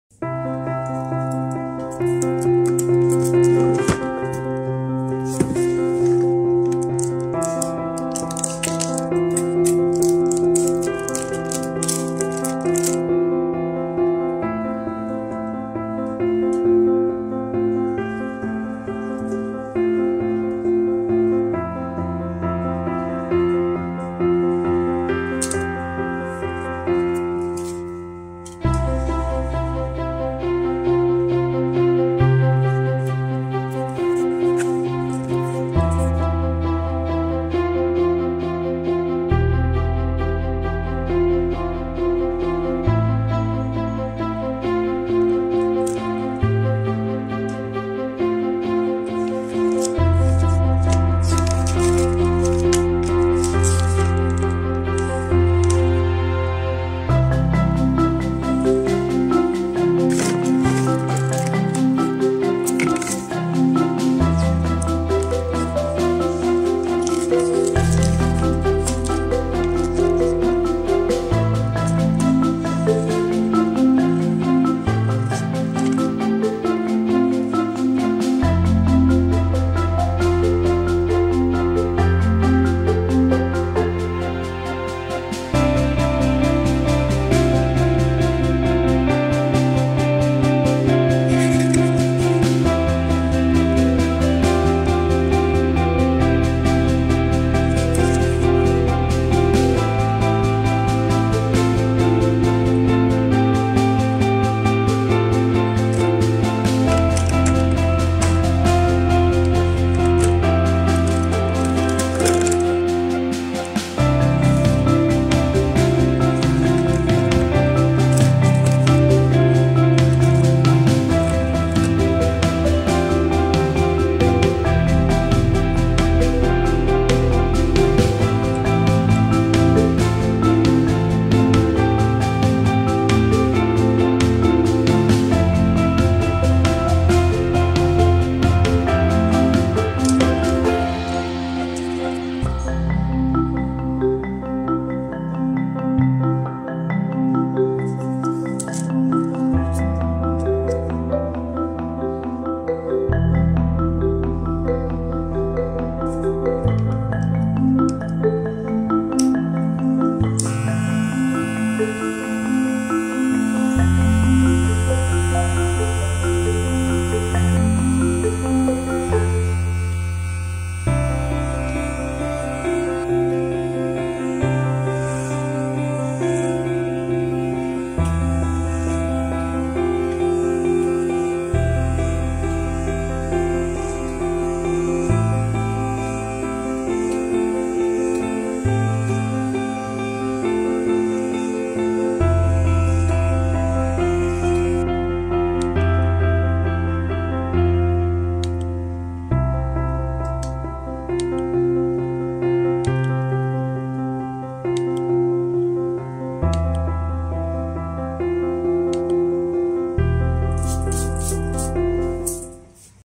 Mesin Cukur Baru Pengganti Enchen: Wahl Super Tapper
Akhir nya Enchen lulus 😂. Setelah setahun lebih saya gunakan dan berangsur-angsur semakin tidak tajam, akhirnya saya memutuskan meminang Wahl Super Tapper. Sebenarnya mesin cukur ini lebih cocok digunakan oleh Profesional sebenarnya ya Buddy. Hanya saja, dari pada saya salah memilih, dan hanya bertahan sebentar, serta tanpa suku cadang pengganti, lebih baik saya memilih mesin cukur satu ini.

Dibandingkan dengan Enchen, mesin cukur ini memiliki lebih banyak opsi size : 3 mm, 6 mm, 10 mm dan 13 mm. Untuk ketajaman menurut saya sudah cukup  hanya untuk perorangan, keluarga saja bahkan lebih dari cukup. Bulid quality tentu saja jauh lebih baik dibandingkan dengan Enchen ya Buddy. Dan walaupun menggunakan kabel  sejauh ini setelah dua bulan lebih pemakaian, saya cukup puas.

Oh ya. jangan tergiur dengan harga yang murah ya Buddy karena banyak versi PALSU yang beredar di pasaran. Harga waktu saya beli berkisar 600 ribu an (diskonan yaaaa... 😂)